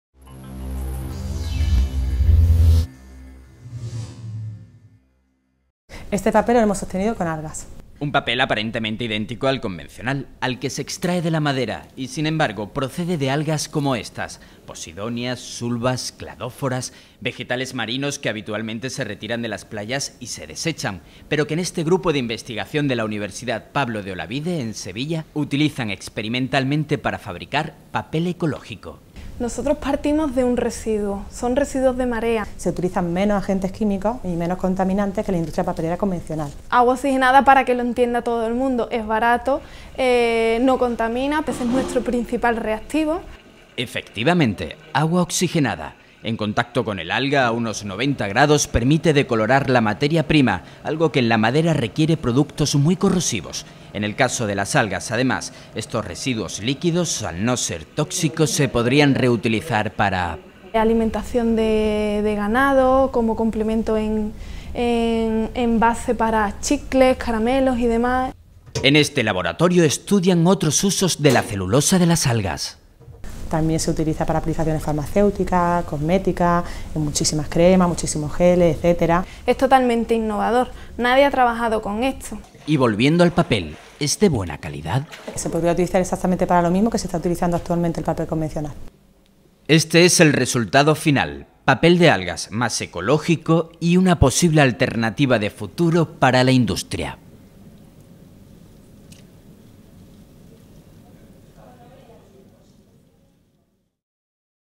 HDL Papel a partir de celulosa de algas marinas, alternativa sostenible a la industria convencional
Es un proyecto del grupo de investigación ECOWAL, de la Universidad Pablo de Olavide de Sevilla. Utilizan distintas especies de algas y prescinden de productos altamente contaminantes usados en la producción del papel convencional. El papel resultante tiene características similares al que se obtiene de la madera. Además, estos científicos estudian posibles aplicaciones de la celulosa de algas en la industria farmacéutica y cosmética.